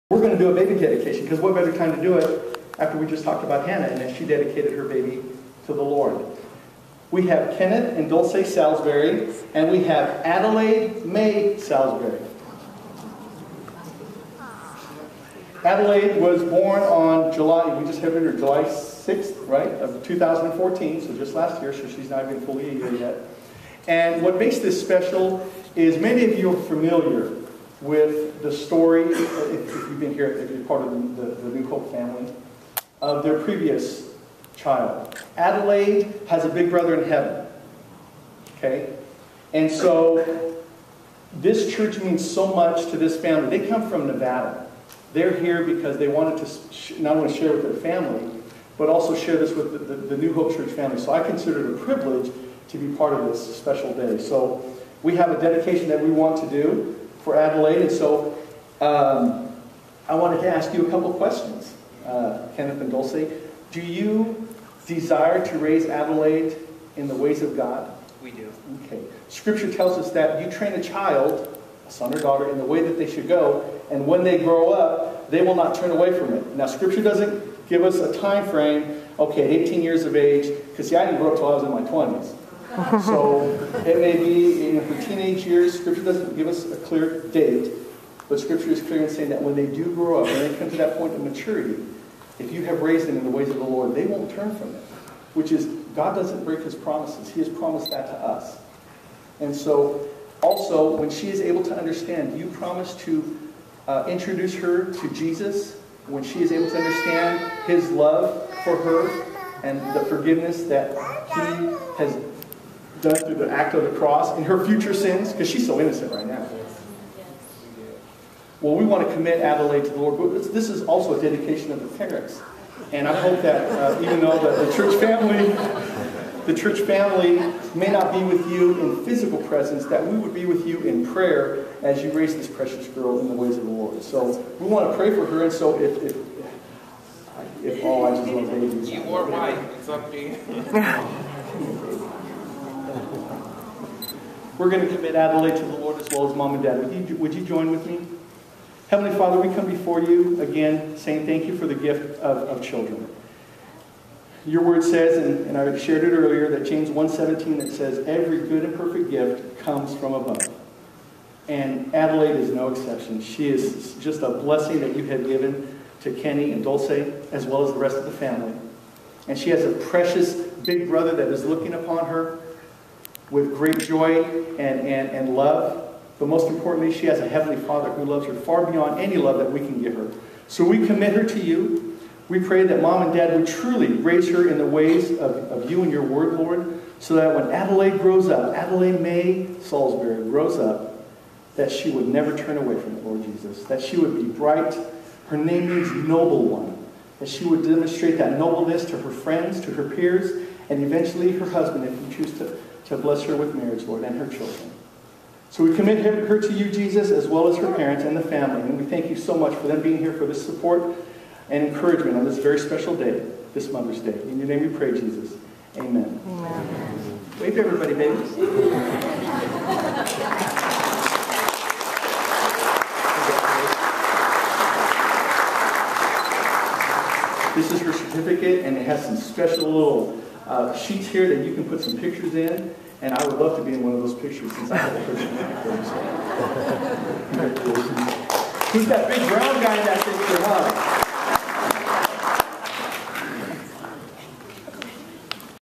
Adelaide's Baby Dedication
On May 10th, 2015 we dedicated Adelaide at New Hope Community Church.  Pastor Nick Delgado gave an amazing service showcasing mothers in the Bible.  We wish each and every one of our family members could have been there, but luckily we have YouTube.